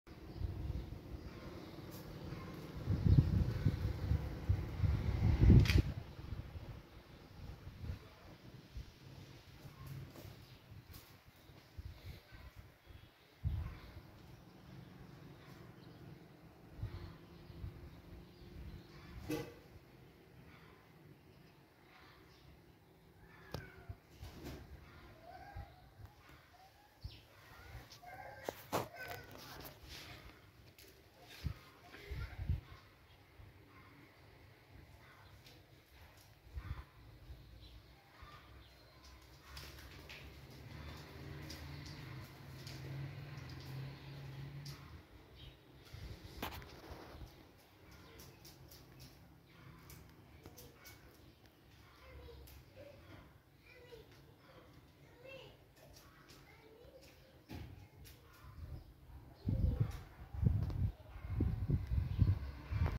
Whiskers in Action: Playful Cat's Fun-filled Adventure! 🐾😸"
Welcome to a thrilling world of feline excitement! In this captivating YouTube video, witness the sheer joy and entertainment that unfolds as our adorable cat indulges in an energetic play session.

Watch in awe as this furry little dynamo leaps, pounces, and romps around with contagious enthusiasm. From chasing feathers to swatting at toy mice, every movement is filled with boundless energy and nimble agility. You'll be on the edge of your seat as our playful feline showcases impressive acrobatics and lightning-fast reflexes.